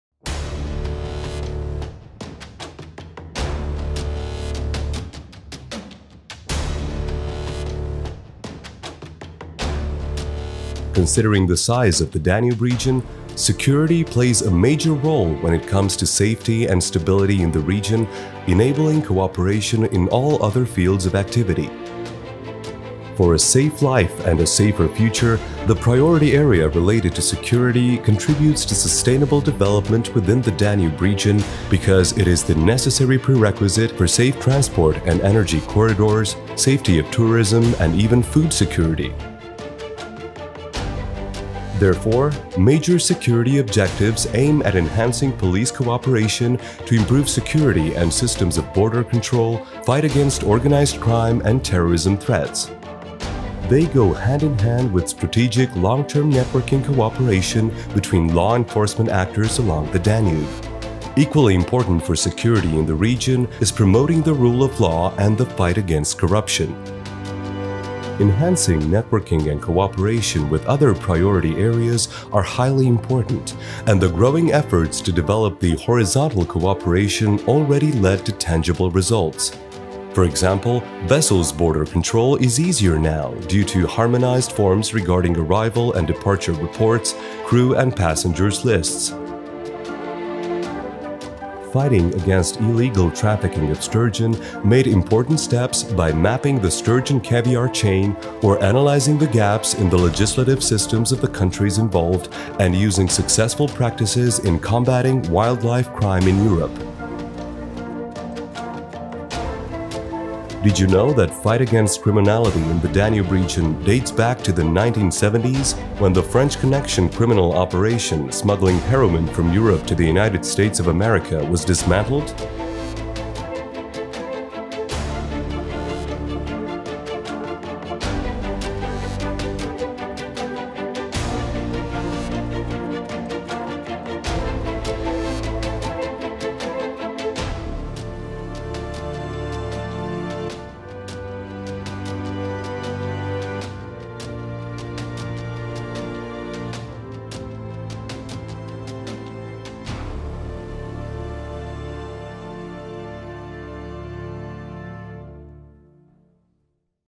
PA11 A SAFER REGION
Considering the size of the Danube Region, security plays a major role when it comes to safety and stability in the region, enabling cooperation in all other fields of activity.
For a safe life and a safer future, the priority area related to security contributes to sustainable development within the Danube Region, because it is the necessary prerequisite for safe transport and energy corridors, safety of tourism and even food security.
Therefore, major security objectives aim at enhancing police cooperation to improve security and systems of border control, fight against organised crime and terrorism threats. They go hand in hand with strategic long-term networking cooperation between law enforcement actors along the Danube. Equally important for security in the region is promoting the rule of law and the fight against corruption.
Enhancing networking and cooperation with other priority areas are highly important and the growing efforts to develop the “horizontal cooperation” already led to tangible results. For example, vessels border control is easier now due to harmonized forms regarding arrival and departure reports, crew and passengers lists.  Fighting against illegal trafficking of sturgeon made important steps by mapping the sturgeon caviar chain or analysing the gaps in the legislative systems of the countries involved and using successful practices in combating wildlife crime in Europe.